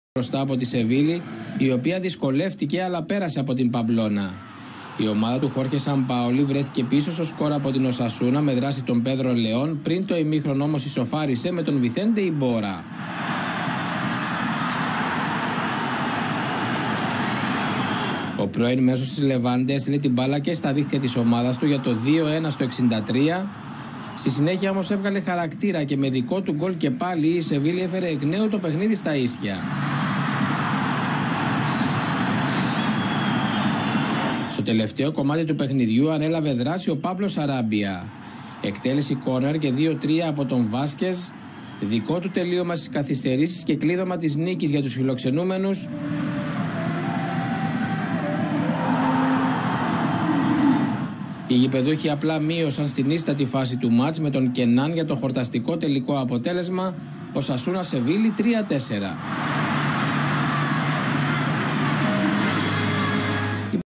Οσασούνα - Σεβίλλη 3-4 /19η αγ. Primera Division {22-1-2017}
ΟΣΑΣΟΥΝΑ: Μάριο, Μπερενγκέρ, Μπονίν, Όιερ, Ου. Γκαρθία, Κλερκ, Τόρες, Τιένθα (88' Κενάν), Τσάουσιτς (74' Ι. Γκαρθία), Λεόν (71' Ριβιέρ), Ριέρα

ΣΕΒΙΛΛΗ: Ρίκο, Μαριάνο, Μερκάντο (65' Σαράμπια), Παρέχα, Ραμί, Εσκουδέρο, Βάσκες (84' Κρανεβίτερ), Ενζονζί, Ιμπόρα, Γιόβετιτς, Μπεν Γεντέρ (90' Κορέα)